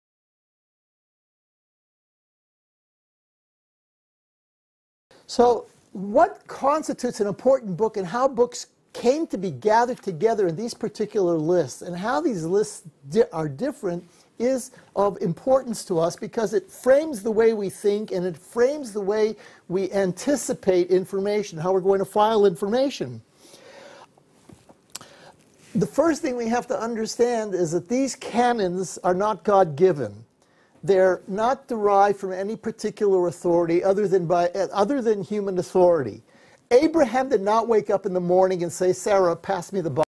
Bible Canon was decided by Human authority, not God - Ziony Zevit
Ziony Zevit (born February 13, 1942) is an American scholar of biblical literature and Northwest Semitic languages and a professor at American Jewish University.

COPYRIGHT